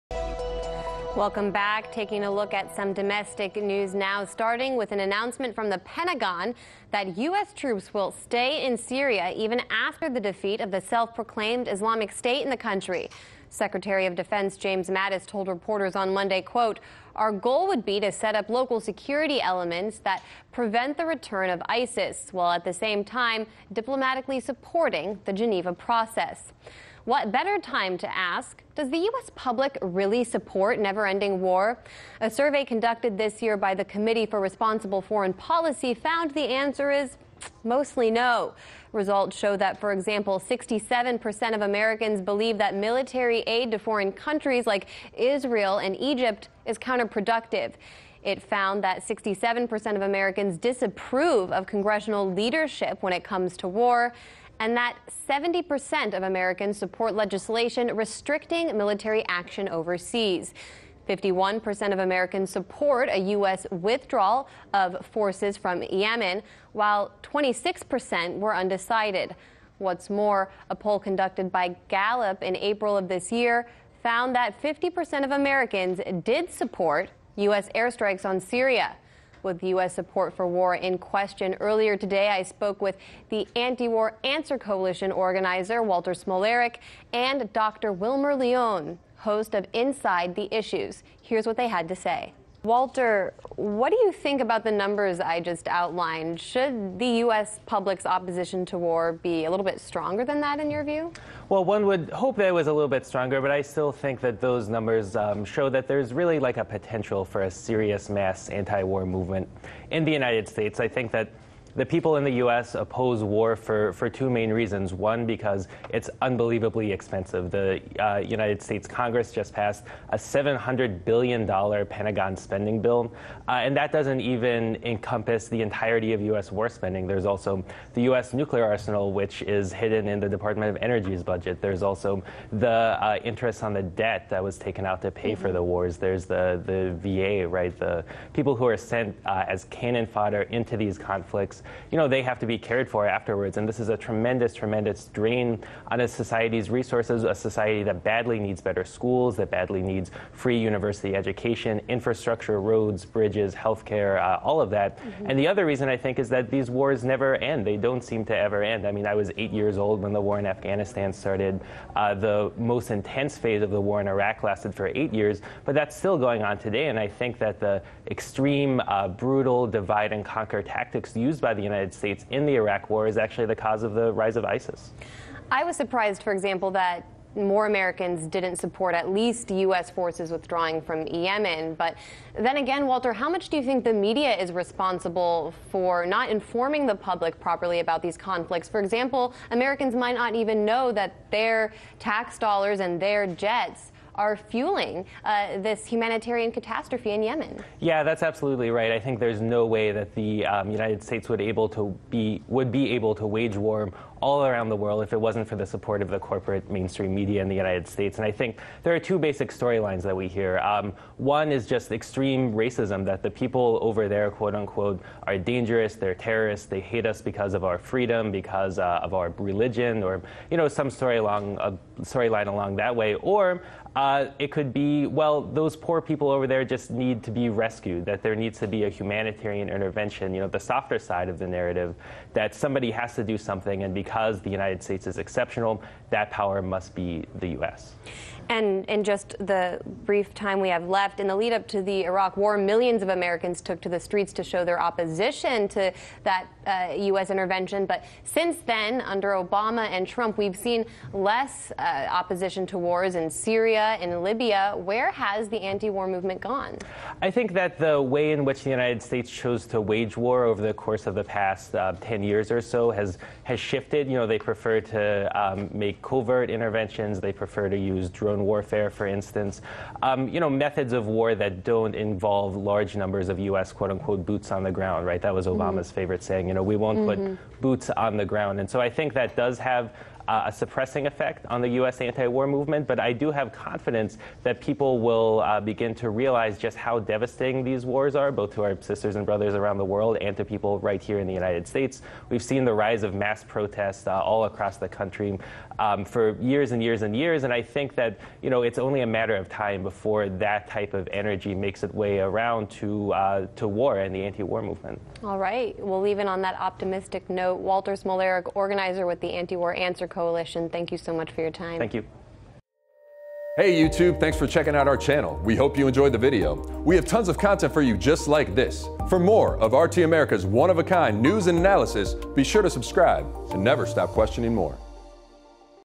Potential For A Mass Anti-War Movement
September 25, 2018 - Anya Parampil examines comments made by General Mattis stating a long-term U.S. military presence in Syria is likely. Anya exposes the contradictory nature of Mattis’ comments when compared to findings in a survey conducted by the Committee for Responsible Foreign Policy suggesting Americans are tired of war. Finally, she talks to ANSWER Coalition organizer Walter Smolarek, about today’s anti-war movement and what working Americans want instead of an inflated war budget.